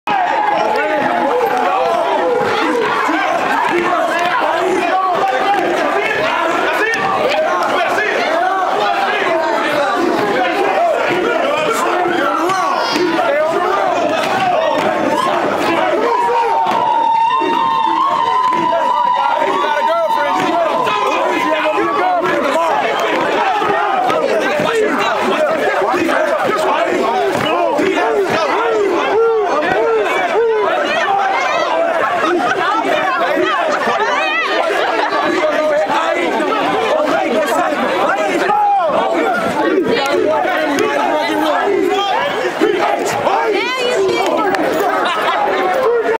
SCSU Omega Psi Phi Death March 2013
This is the Omegas deathmarch and i was only able to record it at this point which is nearly the end of it but enjoy